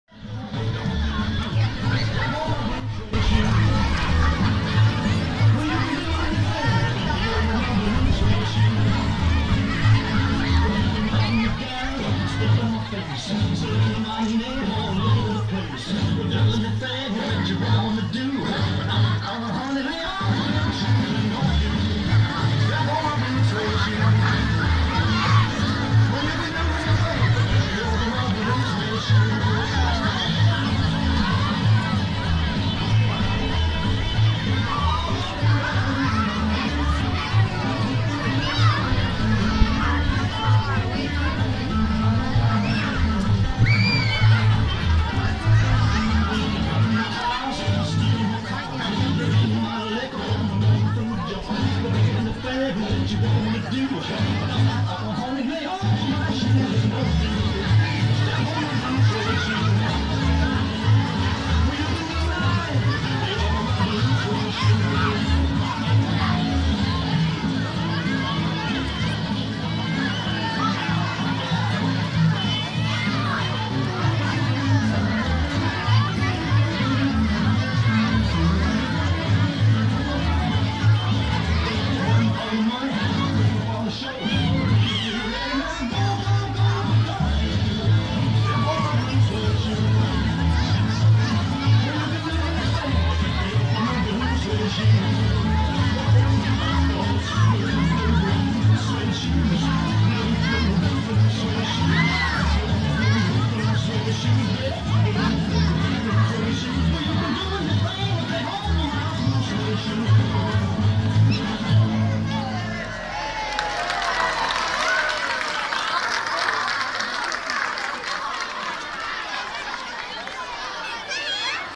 Dansfestijn bij De Heemde
PC De Heemde aan de Hanzelaan in Almelo deed het project 'Kids en Kunst'. De expositie van de resultaten van de leerlingen werd voorafgegaan door een dansfestijn op het schoolplein.